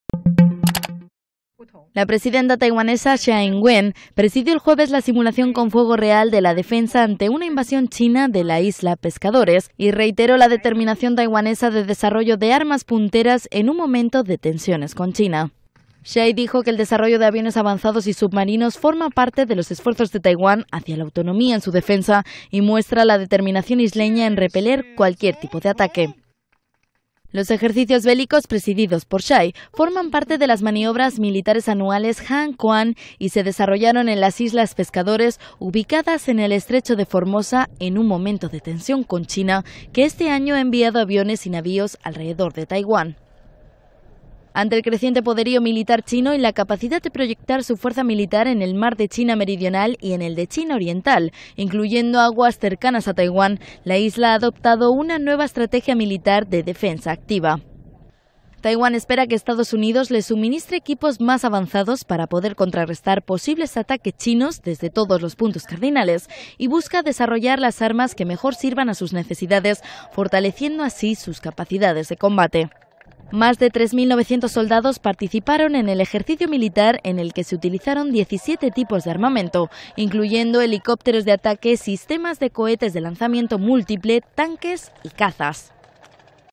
Taiwán realiza la simulación de defensa ante una invasión china
Islas Pescadores (Taiwán), 25 may (EFE/EPA).- La presidenta taiwanesa, Tsai Ing-wen, presidió la simulación con fuego real de la defensa ante una invasión china de la islas Pescadores, y reiteró la determinación taiwanesa de desarrollo de armas punteras, en un momento de tensiones con China.

IMÁGENES CEDIDAS POR LA AGENCIA MILITAR DE NOTICIAS DE TAIWÁN.